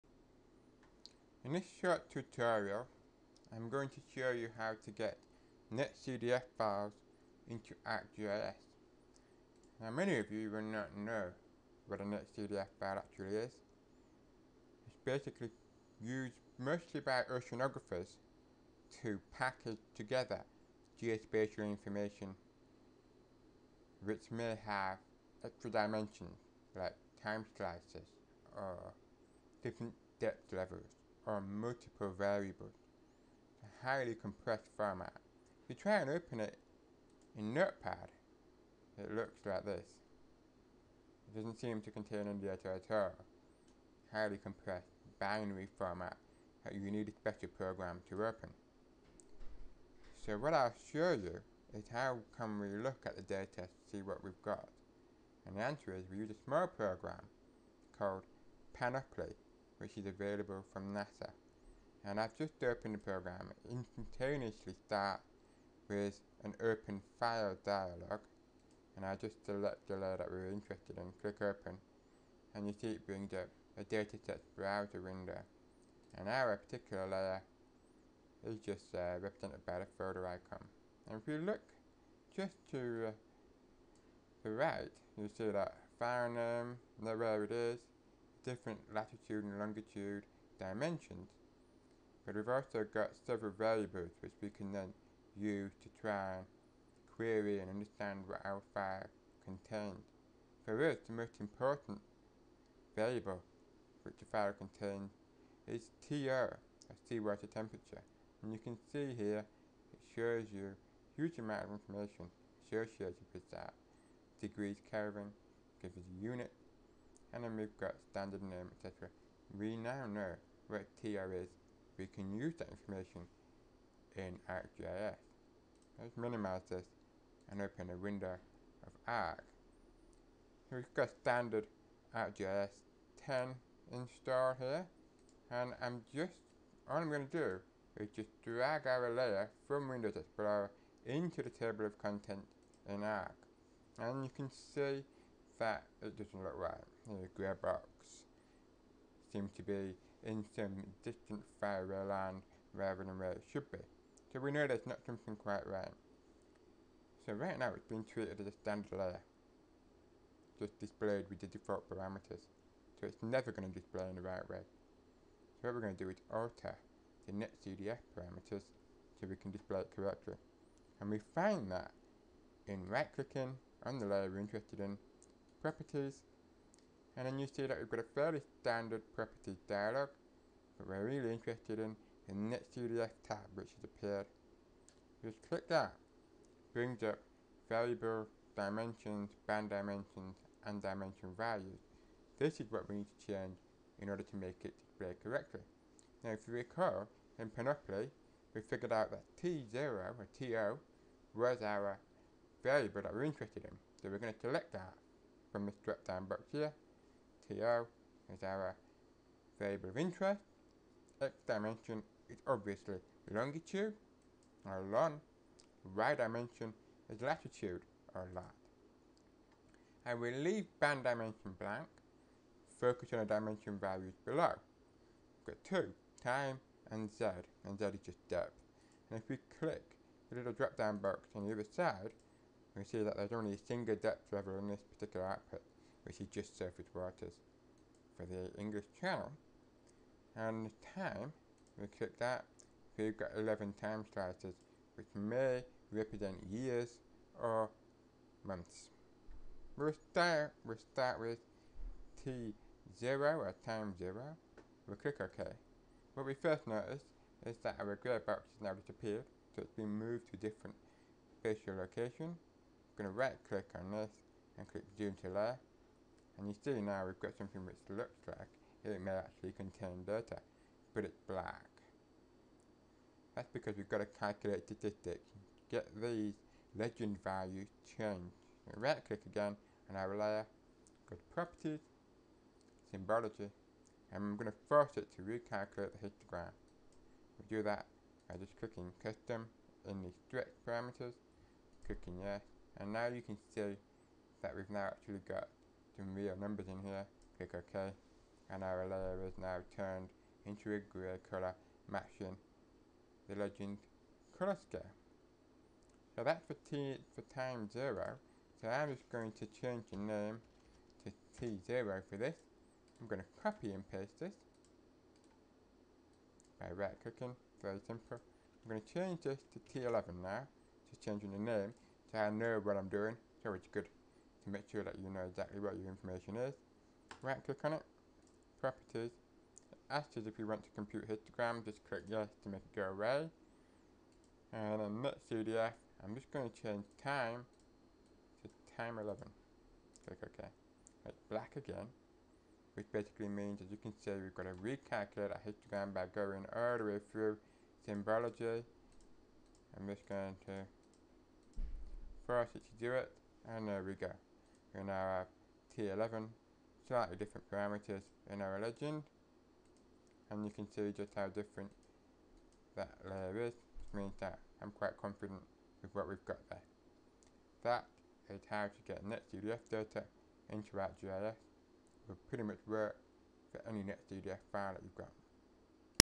Working with a simple NetCDF file in ArcGIS 10
A very simple tutorial for working with a NetCDF file in ArcGIS 10.